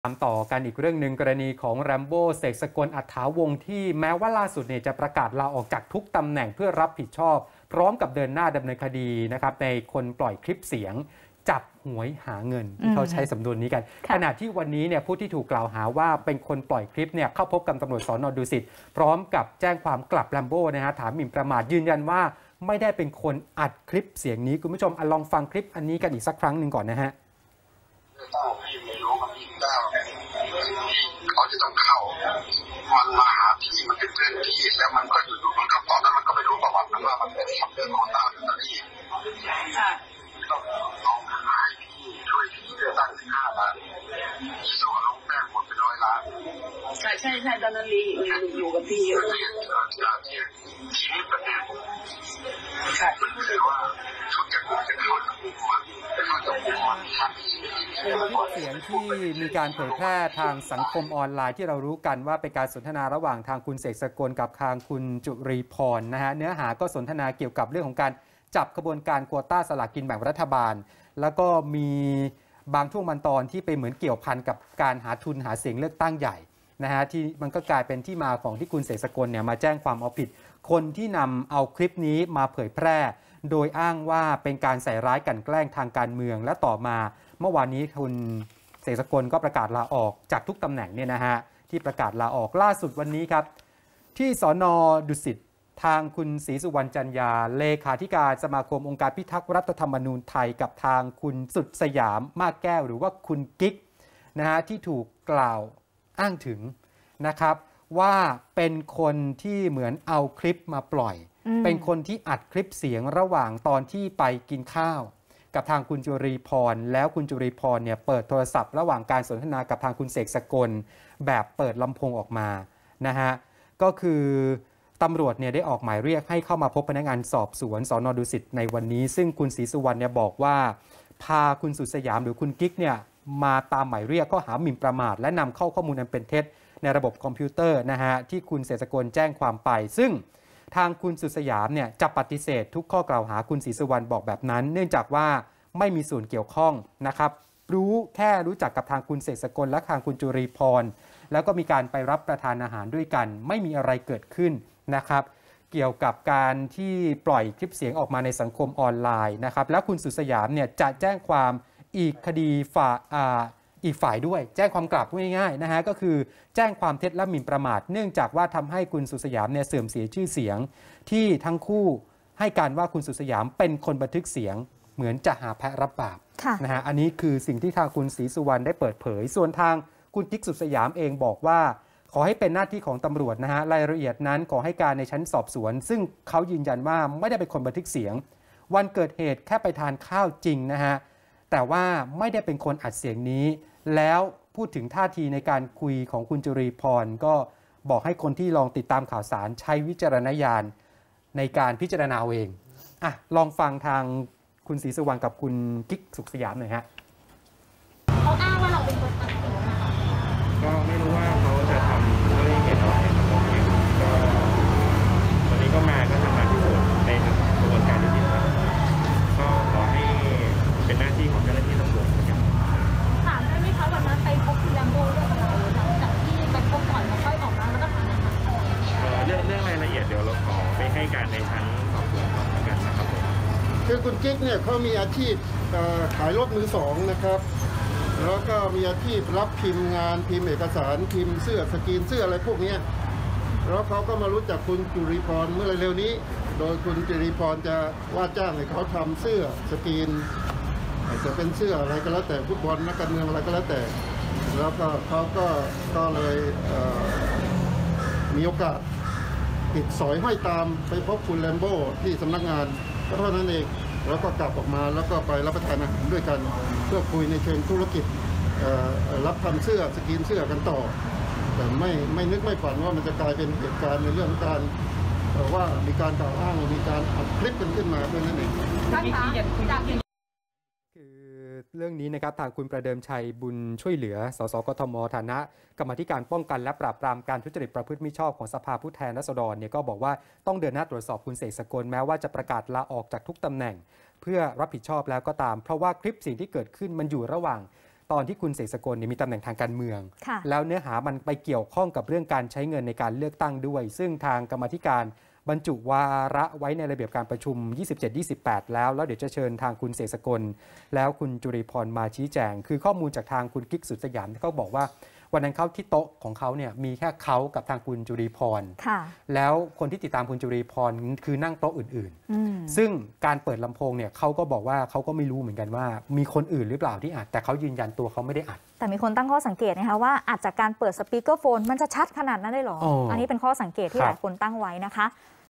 “ศรีสุวรรณ” ควง “สุดสยาม” ให้ปากคำกรณีคลิปเสียงหวยออนไลน์
“ศรีสุวรรณ” พร้อมด้วยนายสุดสยาม มากแก้ว หรือกิ๊ก ให้ปากคำคดีคลิปเสียงหวยออนไลน์ ยืนยันไม่ได้เป็นคนบันทึกเสียงตามที่มีการกล่าวหา พร้อมแจ้งความกลับ “แรมโบ้” ฐานแจ้งความเท็จ หมิ่นประมาท กรณีแพร่คลิปเสียงจับหวยหาเงินเลือกตั้งสมัยที่ผ่านมา